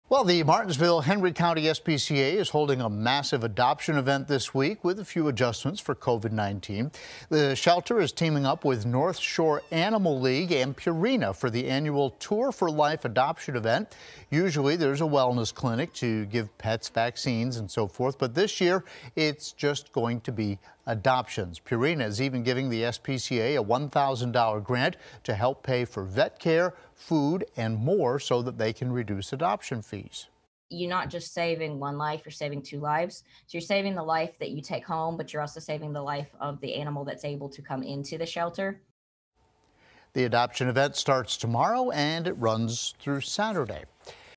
Martinsville-Henry County SPCA holding massive adoption event
Purina is giving the SPCA a $1,000 grand to reduce the adoption fee costs.